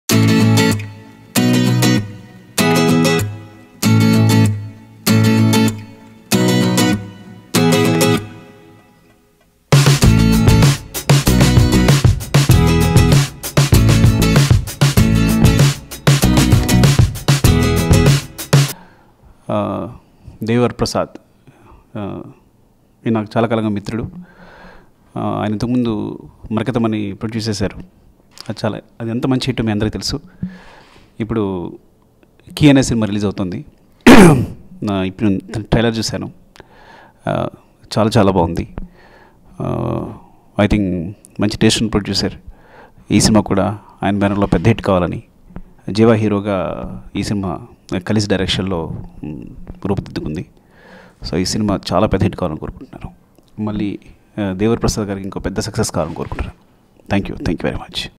Director Sukumar Launch Key 2018 Movie | Rangasthalam 2018 | Tollywood Book
Director Sukumar Launch New Key Movie | Rangasthalam 2018 | Tollywood Book